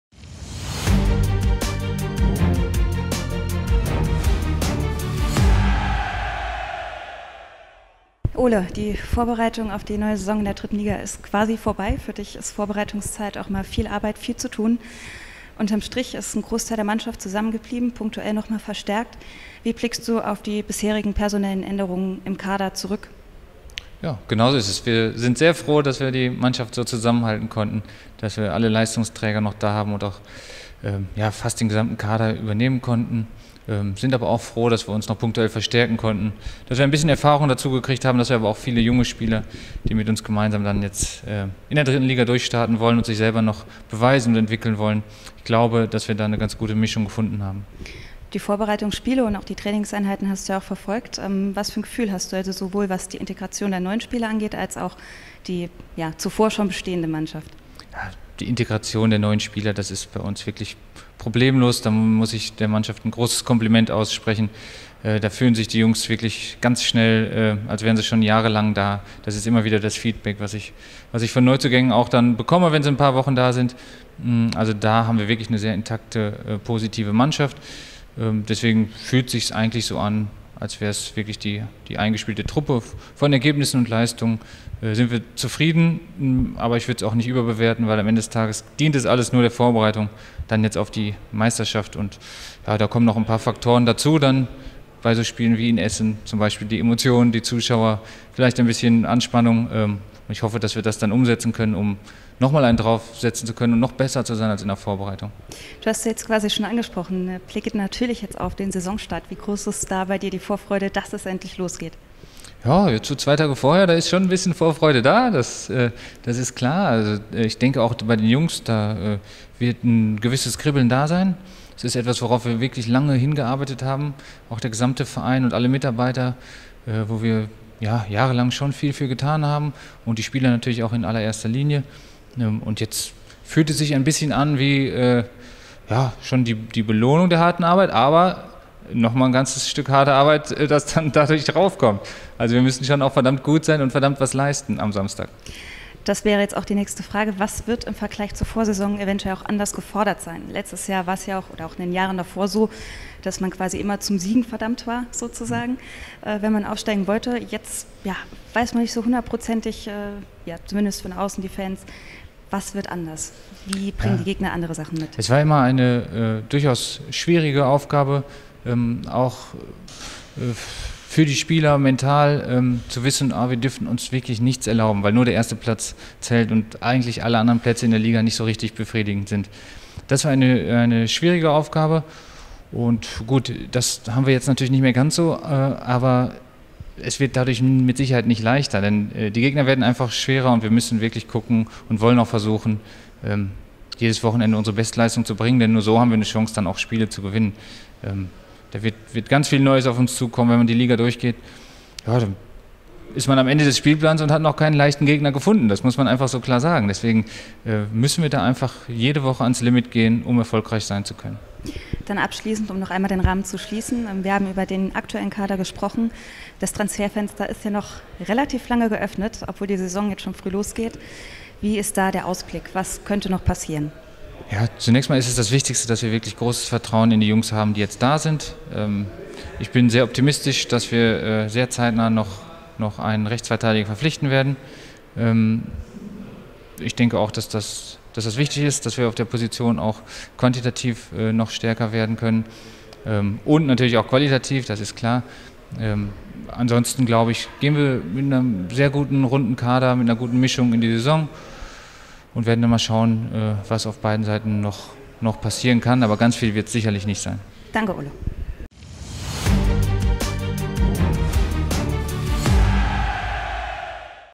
TV Elv // Inside - Interview mit Sportdirektor Ole Book
Vor dem Saison-Start in der 3. Liga haben wir mit Sportdirektor Ole Book über die Kaderplanungen, die Vorbereitung und die anstehende Spielzeit gesprochen.